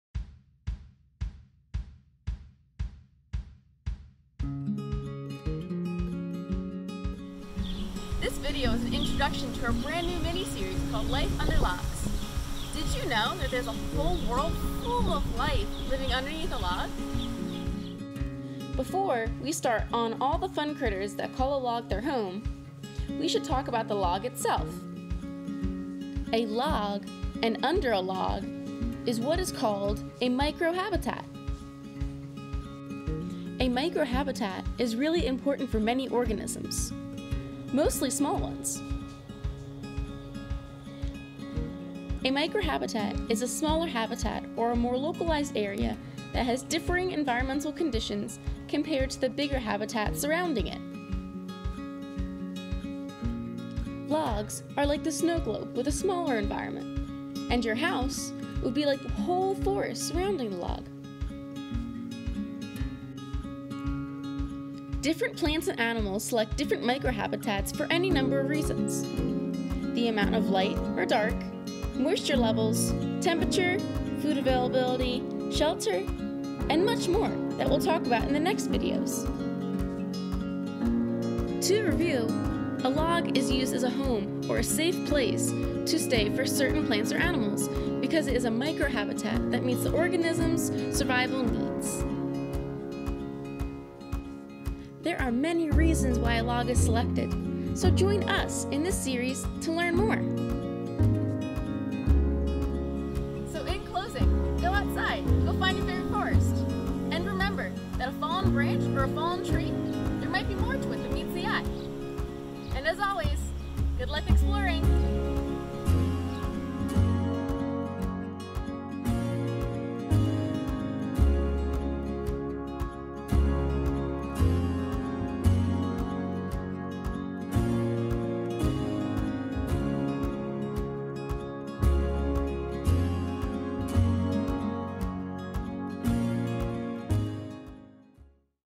Life Under Logs: Microhabitats
Did you know that a fascinating world of life can be living right under a log in a microhabitat? In this 3 part At Home With Nature series, join us to explore the abundant life living right under a log. Watch to learn about the special life that can be hidden away in tiny places.

Learn more about At Home With Nature and explore other Life Under Logs lessons